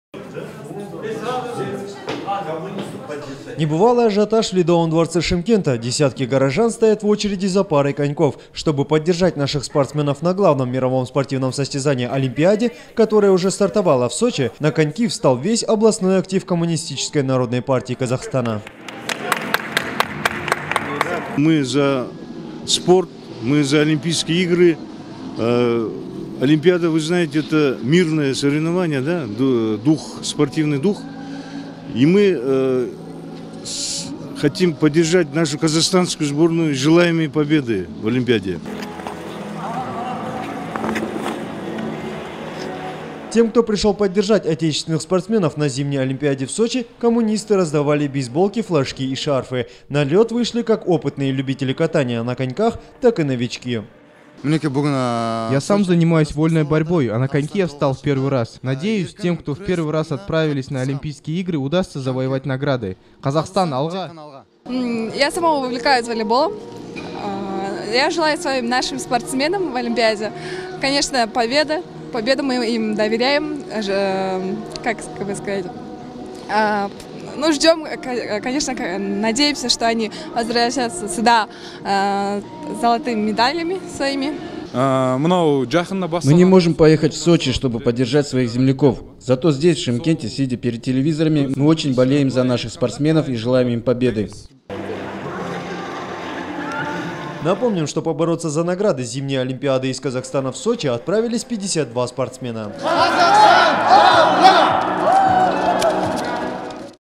В Шымкенте провели массовые катания на коньках в честь Олимпиады в Сочи
Небывалый ажиотаж в Ледовом Дворце Шымкента. Десятки горожан стоят в очереди за коньками. Чтобы поддержать казахстанских спортсменов, принимающих участие в Олимпиаде в Сочи, на лед вышли южно-казахстанские коммунисты.